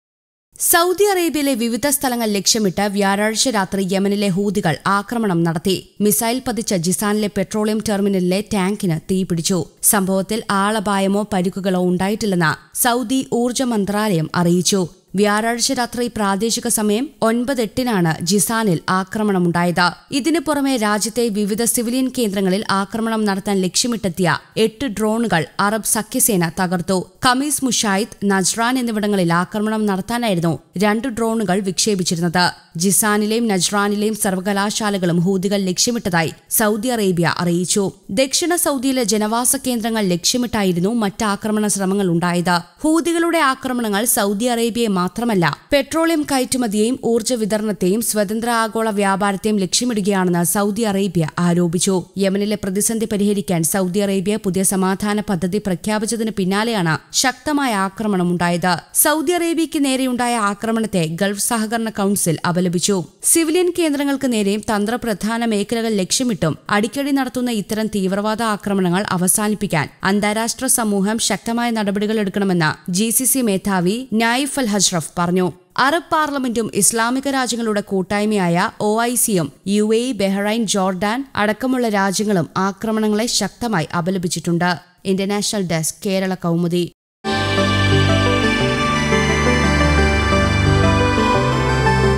Saudi Arabia reports fire at Jizan oil terminal as Houthis escalate attacks | Keralakaumudi
സൗദി അറേബ്യയിലെ വിവിധ സ്ഥലങ്ങള്‍ ലക്ഷ്യമിട്ട് വ്യാഴാഴ്ച രാത്രി യെമനിലെ ഹൂതികള്‍ ആക്രമണം നടത്തി. മിസൈല്‍ പതിച്ച് ജിസാനിലെ പെട്രോളിയം ടെര്‍മിനലിലെ ടാങ്കിന് തീപ്പിടിച്ചു. സംഭവത്തില്‍ ആളപായമോ പരിക്കുകളോ ഉണ്ടായിട്ടില്ലെന്ന് സൗദി ഊര്‍ജ മന്ത്രാലയം അറിയിച്ചു. വ്യാഴാഴ്ച രാത്രി പ്രാദേശിക സമയം രാത്രി 9.08നാണ് ജിസാനില്‍ ആക്രമണമുണ്ടായത്. ഇതിന് പുറമെ  രാജ്യത്തെ വിവിധ സിവിലിയന്‍ കേന്ദ്രങ്ങളില്‍ ആക്രമണം നടത്താന്‍ ലക്ഷ്യമിട്ടെത്തിയ എട്ട് ഡ്രോണുകള്‍ അറബ് സഖ്യസേന തകര്‍ത്തു. ഖമീസ് മുശൈത്ത്, നജ്റാന്‍ എന്നിവിടങ്ങളില്‍ ആക്രമണം നടത്താനായിരുന്നു രണ്ട് ഡ്രോണുകള്‍ വിക്ഷേപിച്ചിരുന്നത്. ജിസാനിലെയും നജ്രാനിലെയും സര്‍വകലാശാലകളും ഹൂതികള്‍ ലക്ഷ്യമിട്ടതായി സൗദി അറേബ്യ അറിയിച്ചു. ദക്ഷിണ സൗദിയിലെ ജനവാസ കേന്ദ്രങ്ങള്‍ ലക്ഷ്യമിട്ട് ആയിരുന്നു മറ്റ് ആക്രമണ ശ്രമങ്ങളുണ്ടായത്.